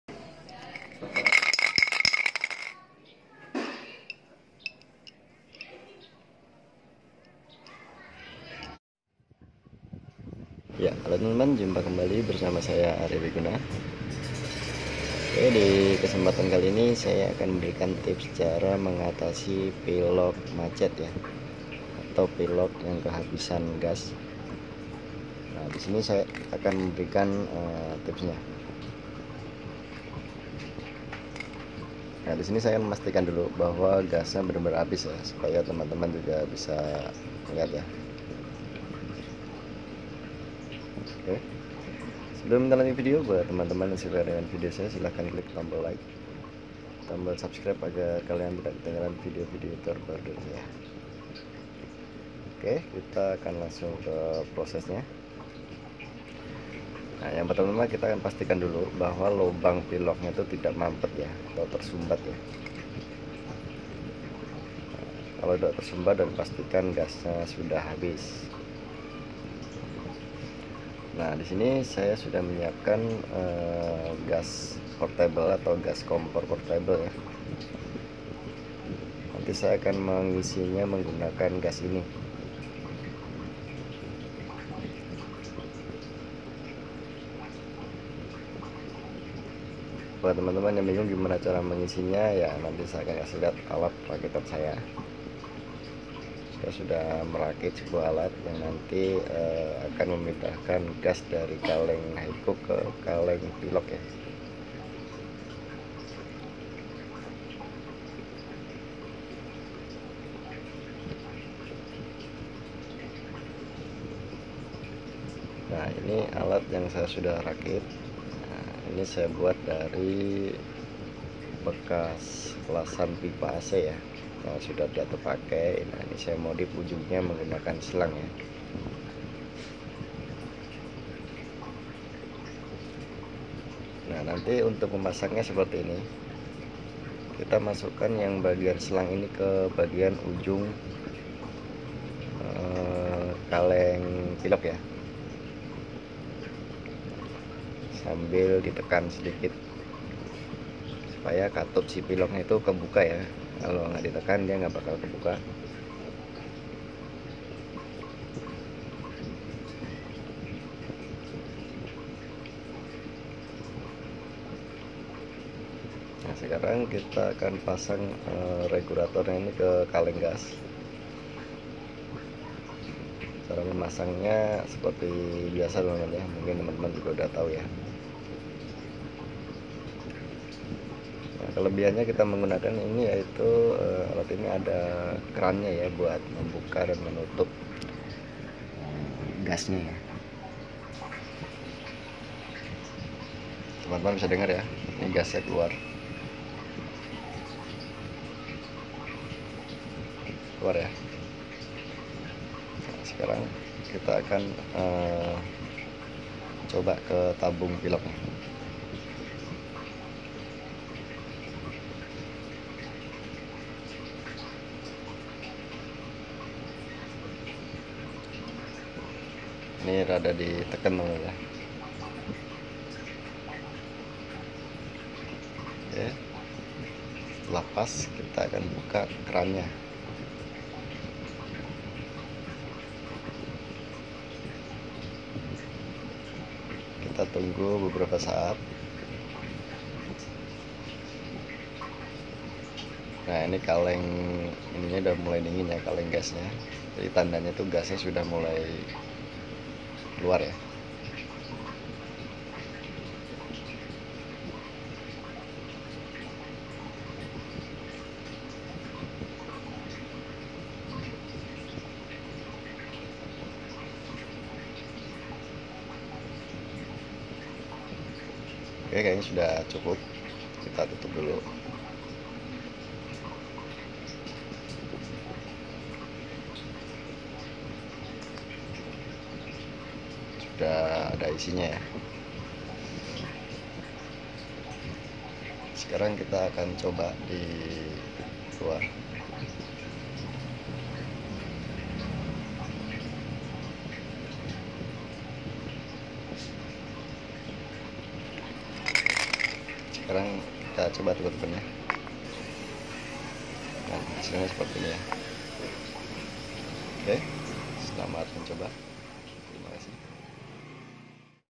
Cara isi ulang pilox tanpa merusak kaleng!!
Trik cara mengatasi pilox macet.
Cara isi ulang pilox
Pilox kehabisan gas

Semoga bermanfaat 🙏